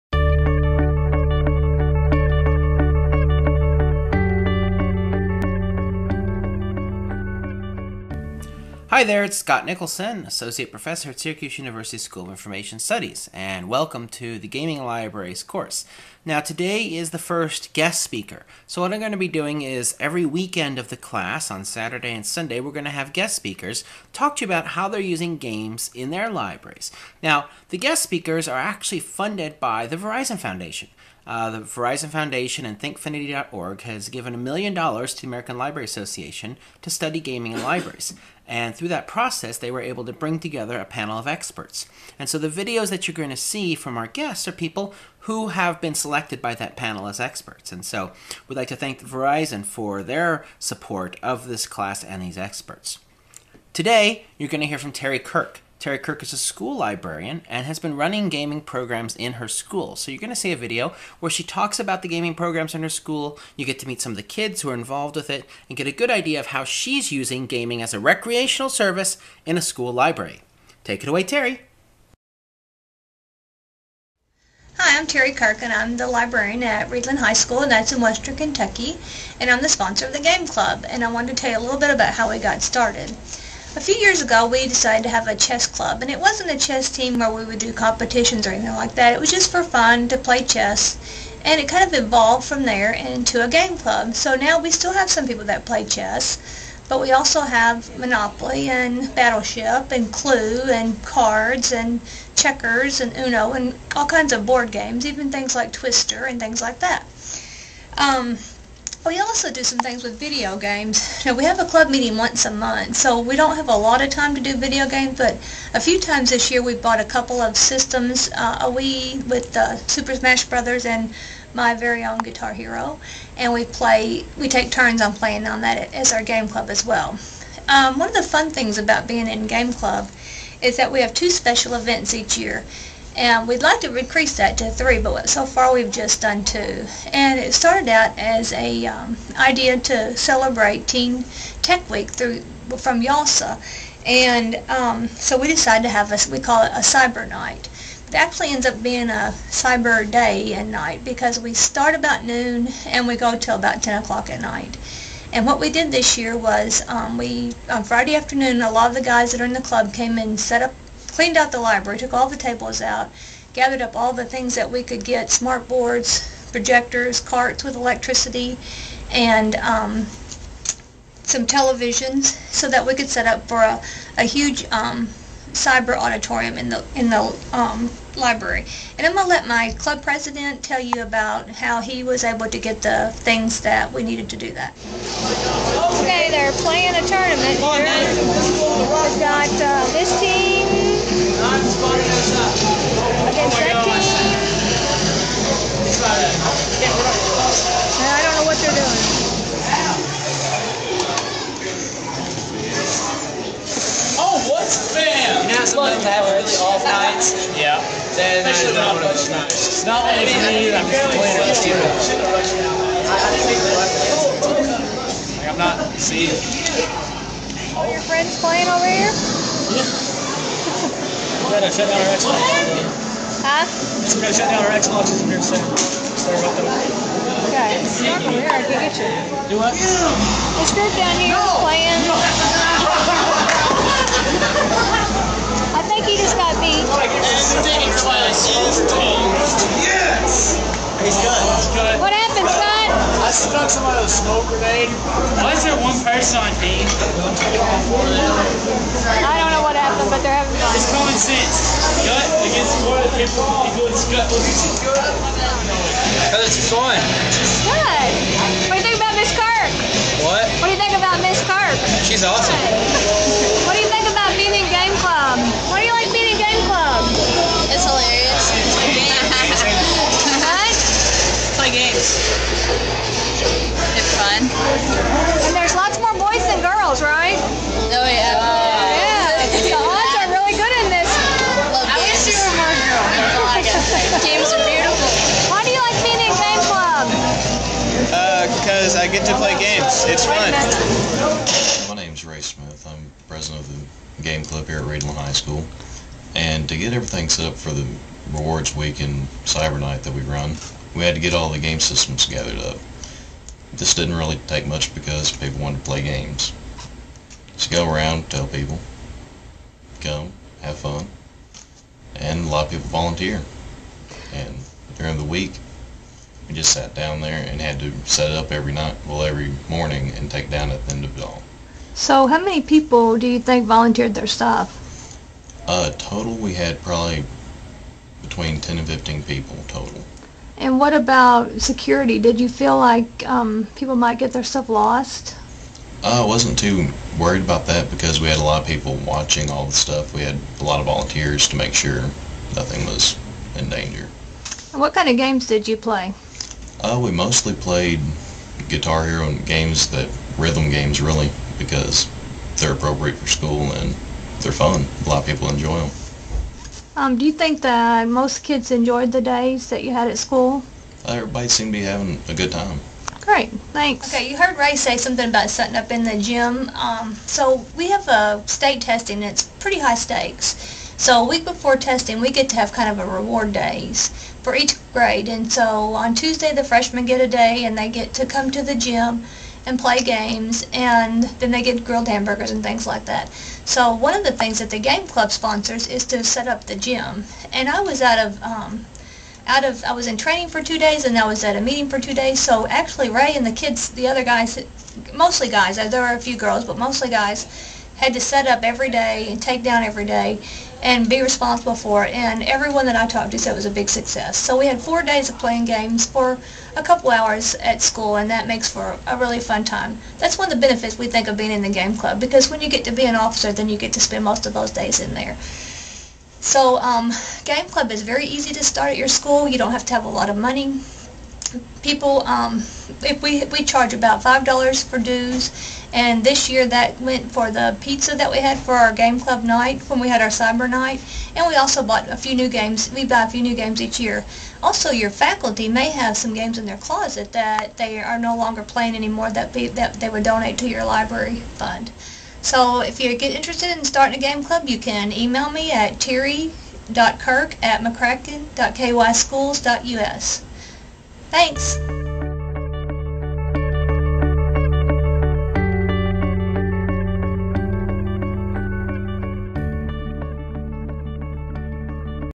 Gaming in Libraries Course Session 5a- Guest Speaker Terri Kirk
Guest Speaker Terri Kirk is a school librarian and has made a video for us about gaming at her school library.

For more information, you can see the entire Gaming in Libraries course, created by Scott Nicholson, or read Everyone Plays at the Library, the book that was inspired by the course.